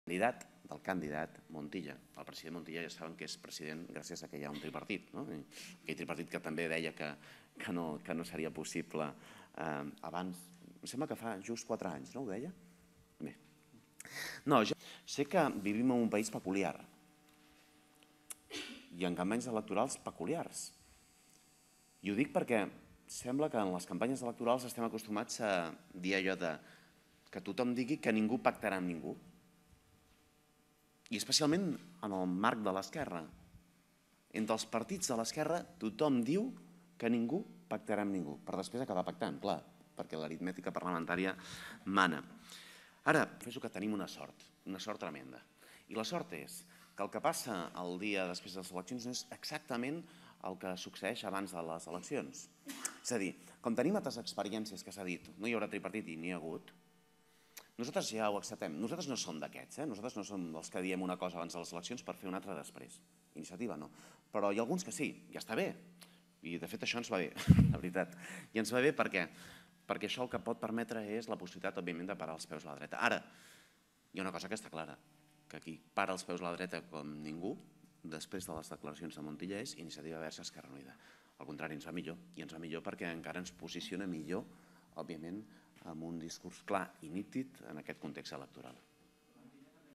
ICV s'erigeix com l'únic partit que "plantarà cara a la dreta"
El cap de llista d'ICV-EUiA a les eleccions del 28-N, Joan Herrera, s'ha erigit avui com l'única força política que plantarà cara a la dreta després de les declaracions del candidat a la presidència de la Generalitat del PSC, José Montilla. "Després de les declaracions de Montilla hi ha més gent que sap que per plantar cara a la dreta la força de referència és iniciativa", ha afirmat Herrera alhora que ha expressat la voluntat de "tenir més força per garantir amb els nostres  vots que es plantar cara a la dreta i que després de les eleccions es duguin a terme polítiques socials i no de tisorades per un govern monocolor".